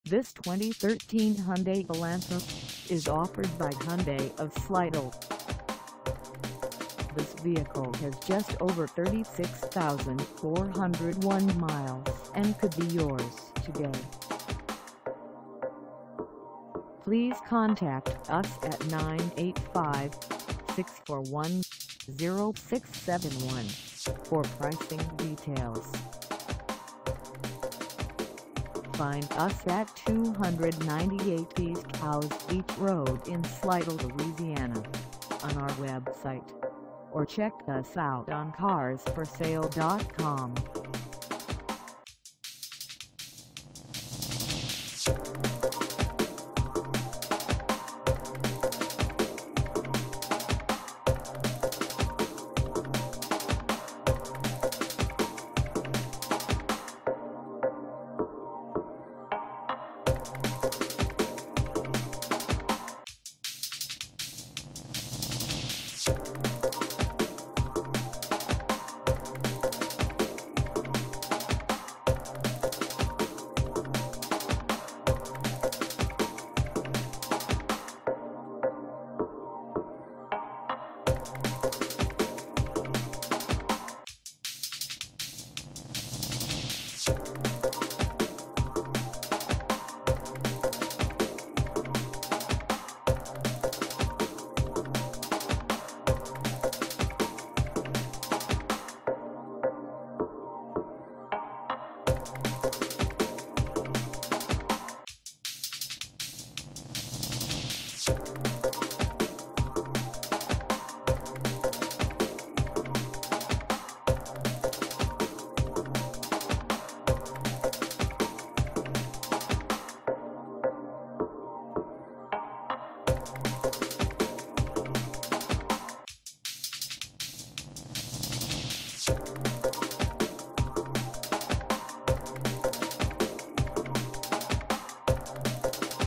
2013 Hyundai Elantra GLS for sale in Slidell, LA 70461 at th
This 2013 Hyundai Elantra GLS is for sale in Slidell, LA 70461 at Hyundai Of Slidell.
Engine: I4 1.8L I4
Transmission: Automatic 6-Speed
Color: Harbor Gray Metallic
Mileage: 36401
Contact Hyundai Of Slidell 298 East Howze Beach Road Slidell, LA 70461 to test drive this 2013 Hyundai Elantra GLS today.